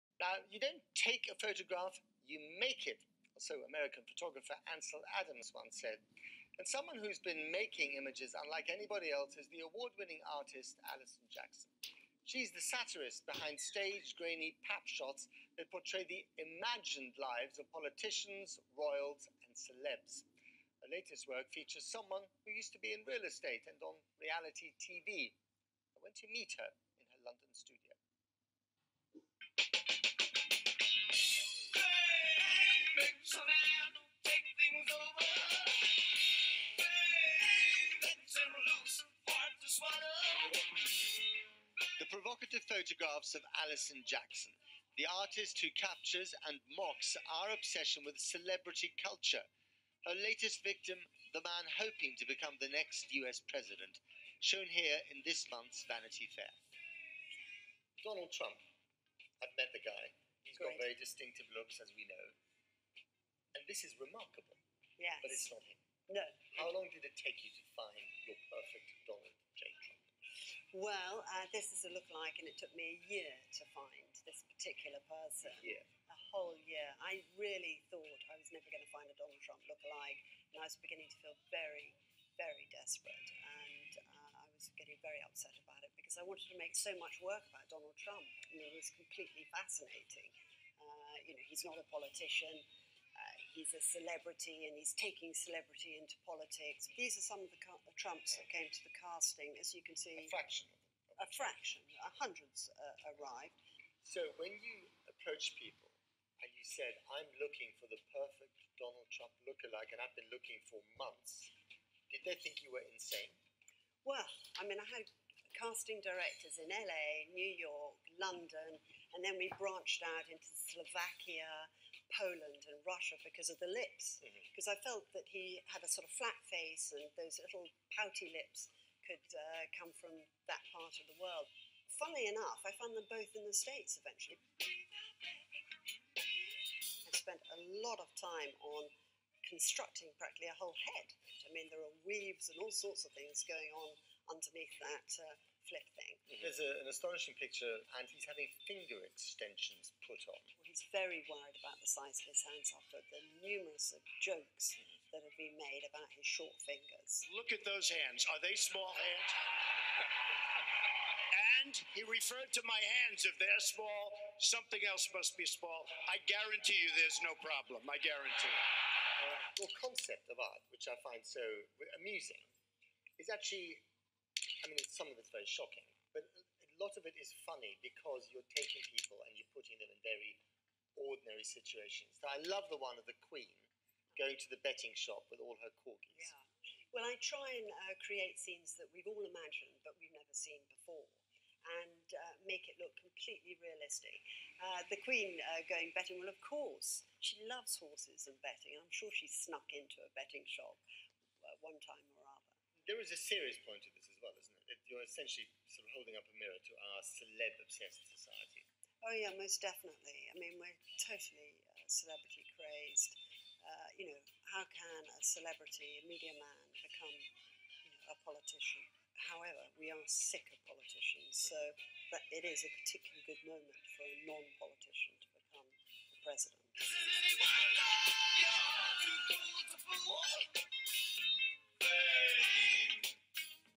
Alison Jackson Channel 4 interview with Matt Frei. We talk Donald Trump images and more!
Alison Jackson Channel 4 Interview with Matt Frei 11/09/16.